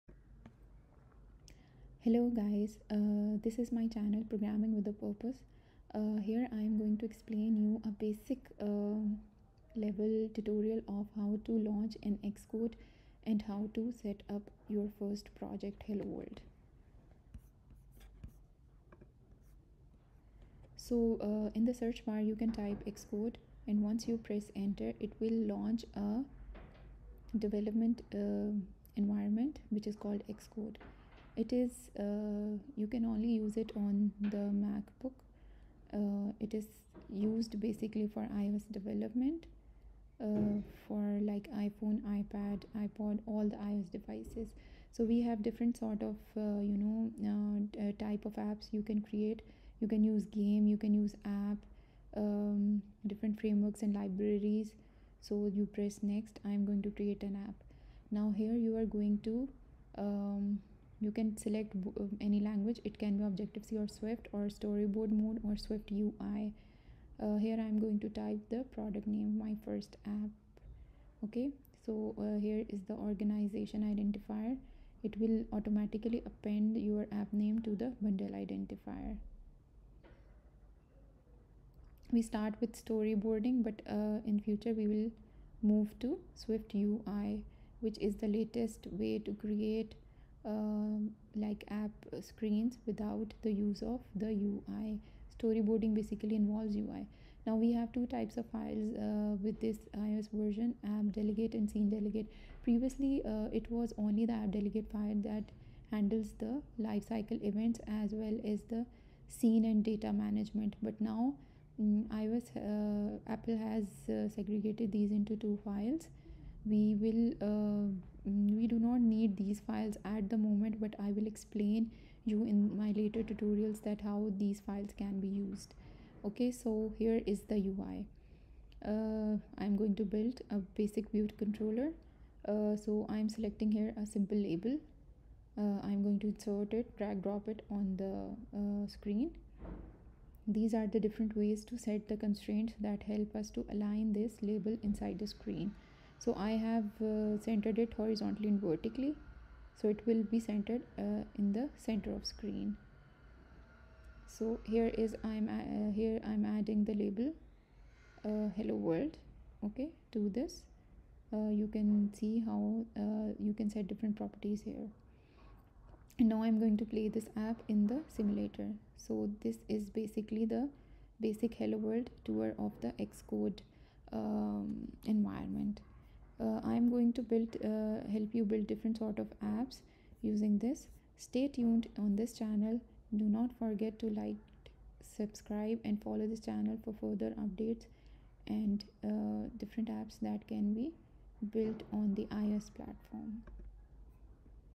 Welcome to iOS Programming: My First iOS App - Hello World in Xcode | iOS Development Tutorial
This video is the introduction of this channel and to xcode environment. Simple example of hello world app with single view controller.